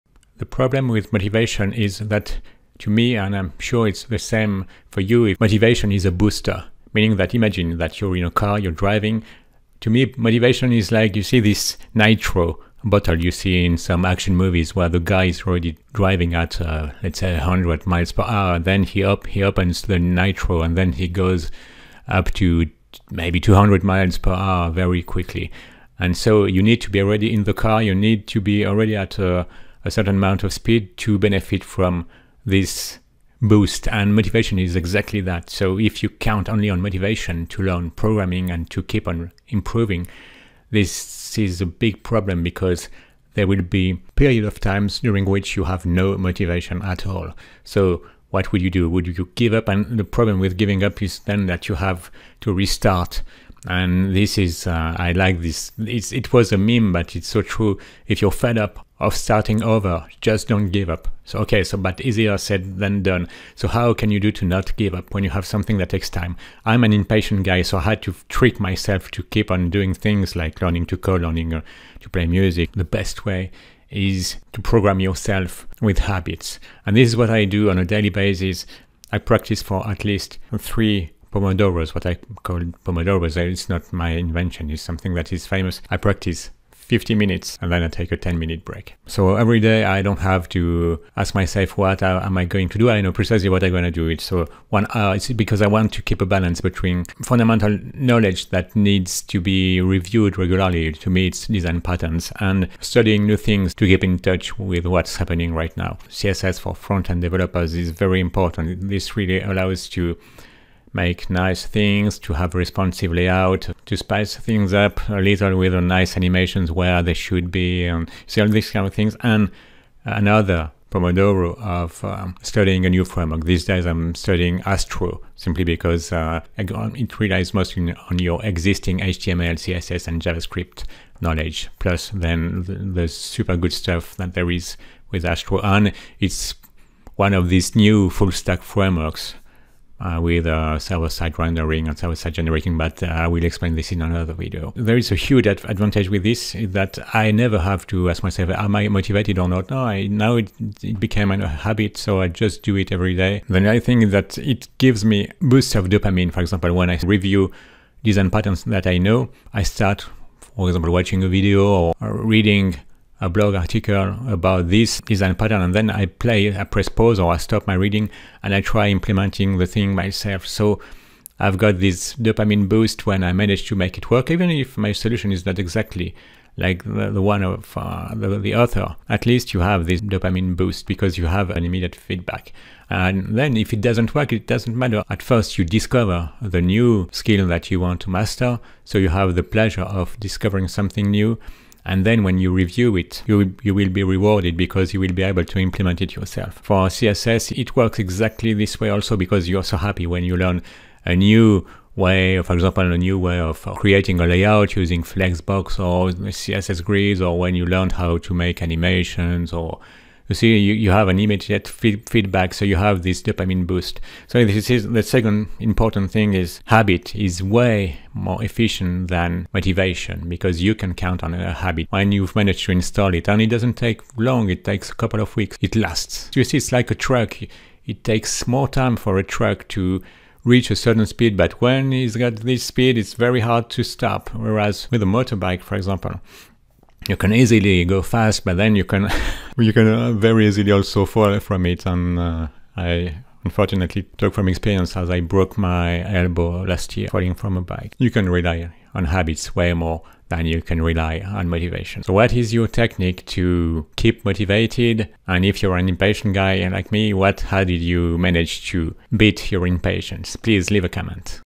How to Keep Coding? Do THIS to become Unstoppable
Web Programming is hard. It requires learning new skills, new libs, new frameworks while consolidating fundamental knowledge (design patterns, data structures etc). You can NOT rely on motivation alone as it would fail you when ... well,  you're not motivated that day / week / month.
In this video, we'll see how to rely on more efficient psychological leverages.

Chapters:
0:00 The problem with motivation
1:45 How to trick your impatience
2:24 Dopamine Oriented Programming